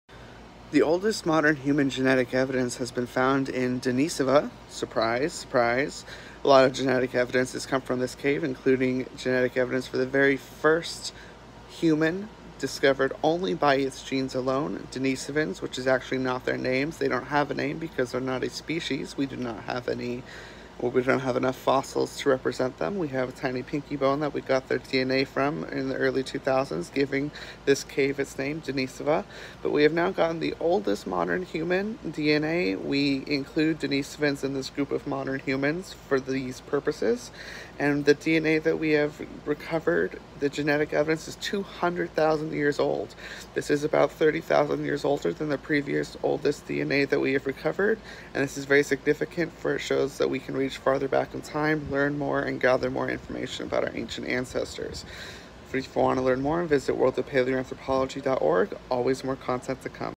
Oldest Human DNA 🧬 Discovered!
“Discover the Oldest Human DNA Ever Found! 🧬 Unearthed in Siberia, this ancient DNA reveals stunning insights into our origins. Watch now to uncover humanity’s deepest roots! HumanOrigins AncientDNA Paleoanthropology Archaeology”